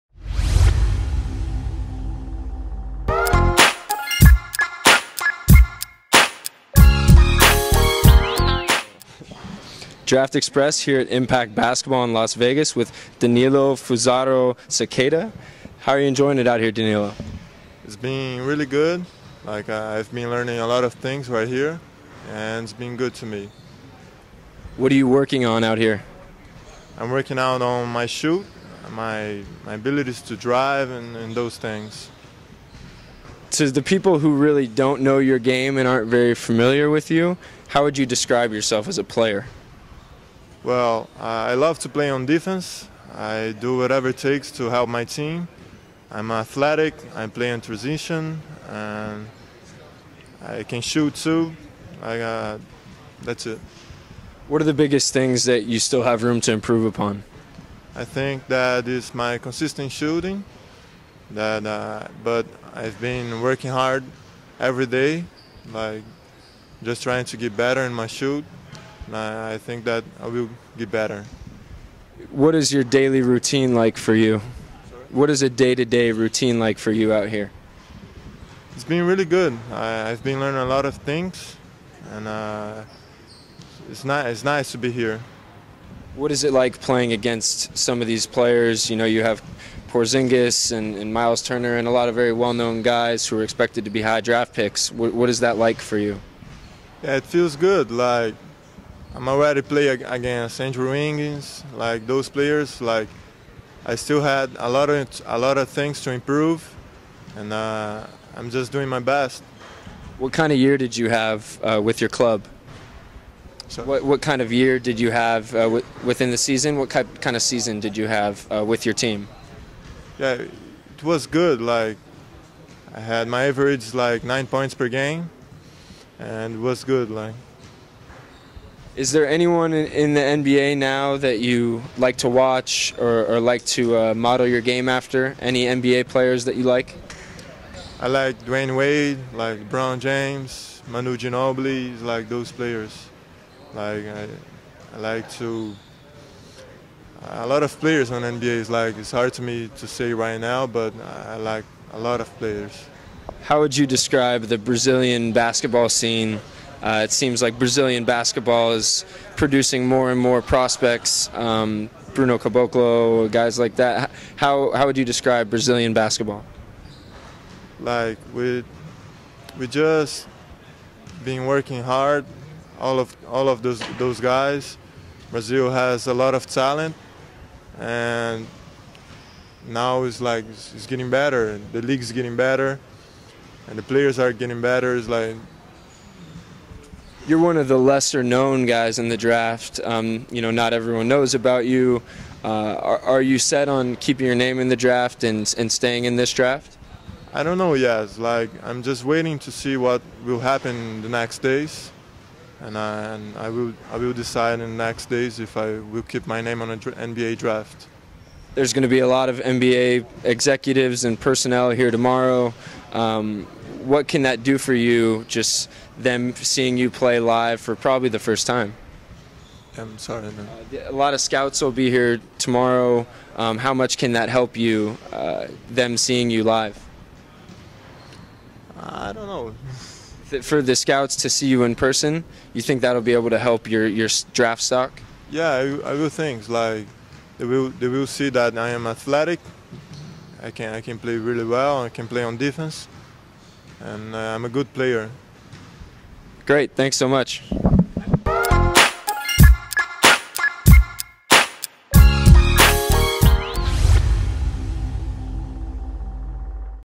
Danilo Fuzaro 2015 ASM Pro Day Interview - DraftExpress
Brazilian guard Danilo Fuzaro sits down with DraftExpress at Impact Basketball in Las Vegas to discuss ASM Pro Day.